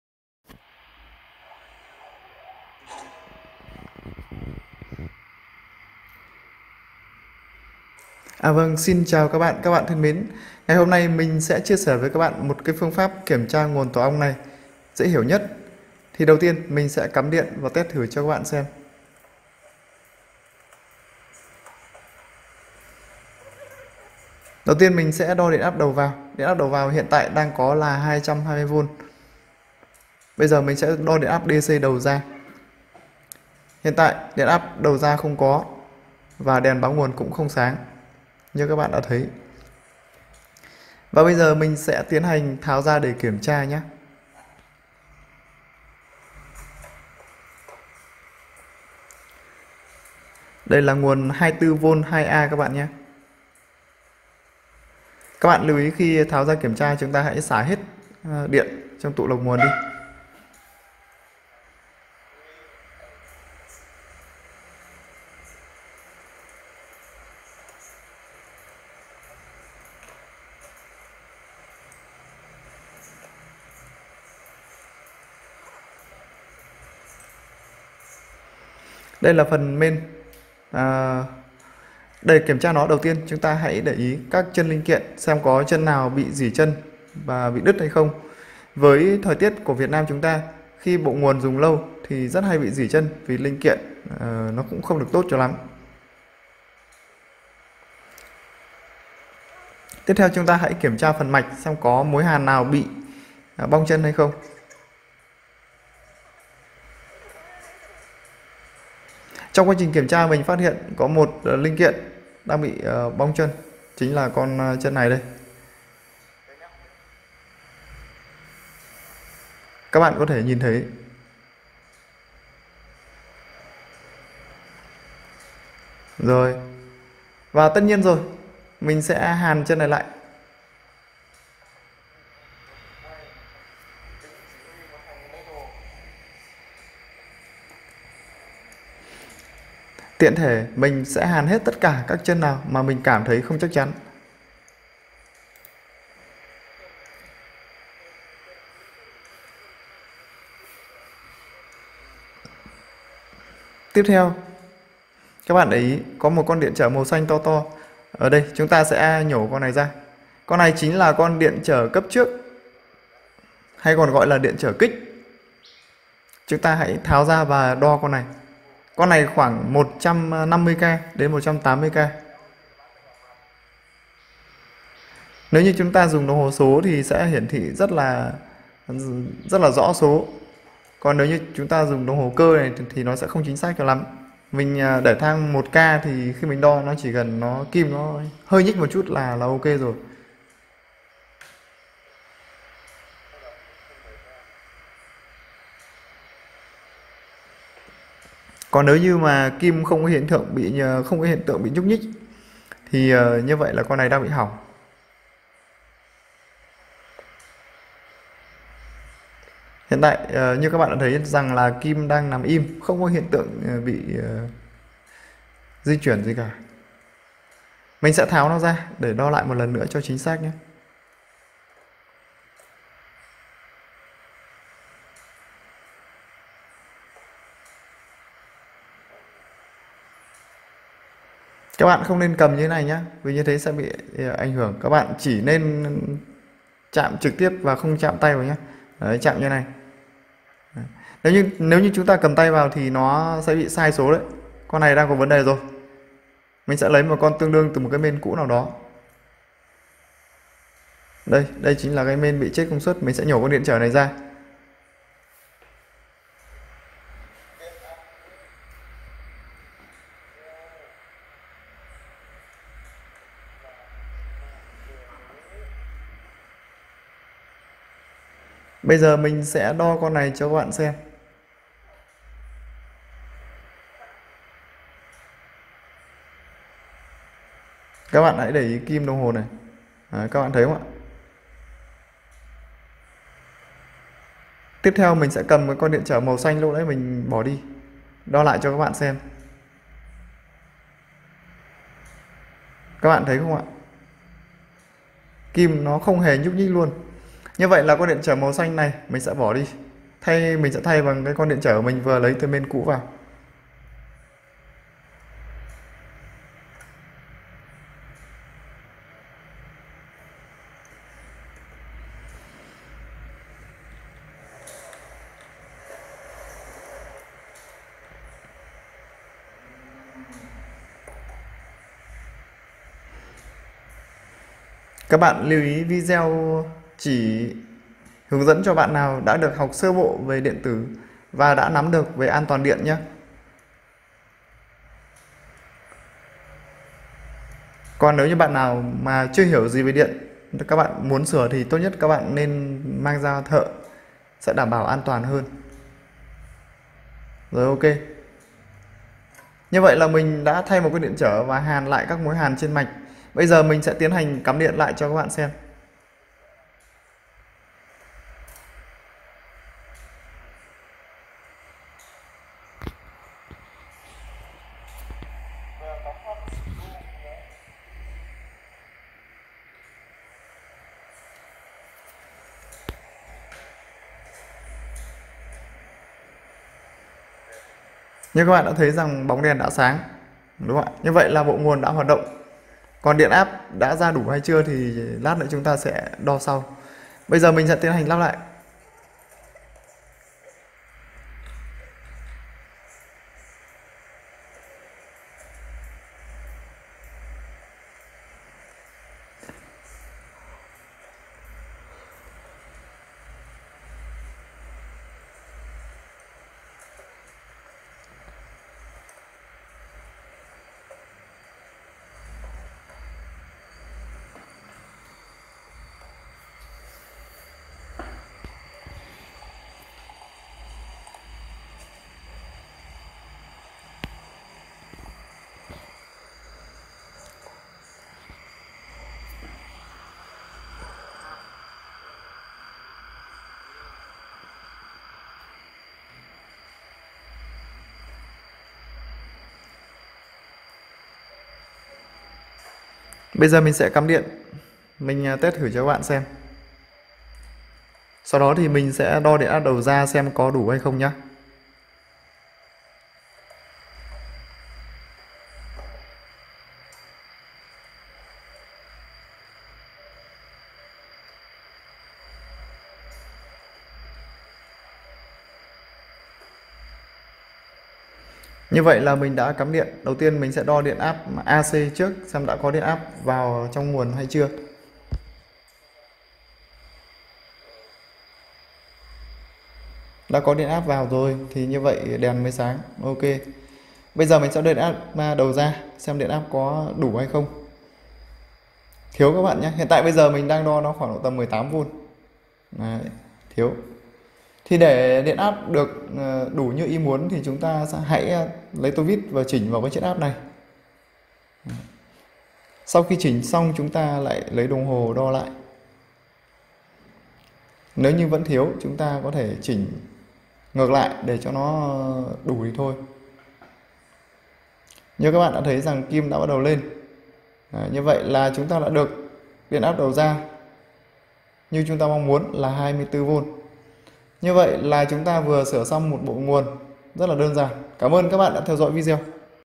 PHƯƠNG PHÁP KIỂM TRA SỬA CHỮA BỘ NGUỒN DỄ HIỂU NHẤT
-Hôm nay mình sẽ cùng kiểm tra sửa chữa bộ nguồn xung nhé.